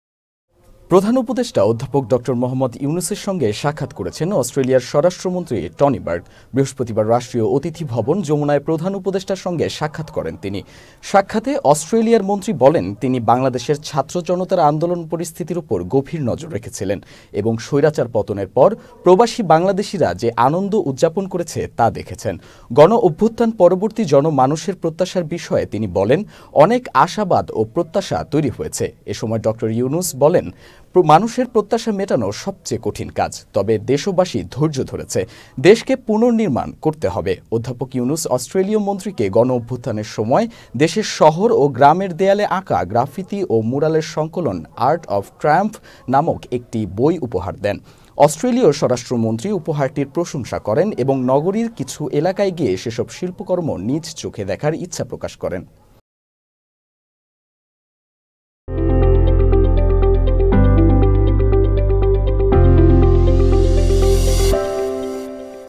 প্রধান উপদেষ্টার সঙ্গে সাক্ষাৎ করেছেন অস্ট্রেলিয়ার স্বরাষ্ট্রমন্ত্রী টনি বার্ক | Channel S news
প্রধান উপদেষ্টা অধ্যাপক ড. মুহাম্মদ ইউনূসের সঙ্গে সাক্ষাৎ করেছেন অস্ট্রেলিয়ার স্বরাষ্ট্রমন্ত্রী টনি বার্ক। বৃহস্পতিবার রাষ্ট্রীয় অতিথি ভবন যমুনায় প্রধান উপদেষ্টার সঙ্গে সাক্ষাৎ করেন তিনি।সাক্ষাতে অস্ট্রেলিয়ার মন্ত্রী বলেন, তিনি বাংলাদেশের ছাত্র-জনতার আন্দোলন পরিস্থিতির ওপর গভীর নজর রেখেছিলেন এবং স্বৈরাচার পতনের পর প্রবাসী বাংলাদেশিরা যে আনন্দ উদযাপন করেছে তা দেখেছেন। গণ-অভ্যুত্থান পরবর্তী জনমানুষের প্রত্যাশার বিষয়ে তিনি বলেন, অনেক আশাবাদ ও প্রত্যাশা তৈরি হয়েছে। এ সময় ড. মুহাম্মদ ইউনূস বলেন, মানুষের প্রত্যাশা মেটানো সবচেয়ে কঠিন কাজ। তবে দেশবাসী ধৈর্য ধরেছে। দেশকে পুননির্মাণ করতে হবে। অধ্যাপক ইউনূস অস্ট্রেলীয় মন্ত্রীকে গণ-অভ্যুত্থানের সময় দেশের শহর ও গ্রামের দেয়ালে আঁকা গ্রাফিতি ও ম্যুরালের সংকলন ‘আর্ট অব ট্রায়াম্ফ’ নামক একটি বই উপহার দেন। অস্ট্রেলীয় স্বরাষ্ট্রমন্ত্রী উপহারটির প্রশংসা করেন এবং নগরীর কিছু এলাকায় গিয়ে সেসব শিল্পকর্ম নিজ চোখে দেখার ইচ্ছা প্রকাশ করেন.

1: News :
2: Entertainment News :
3: Sports News :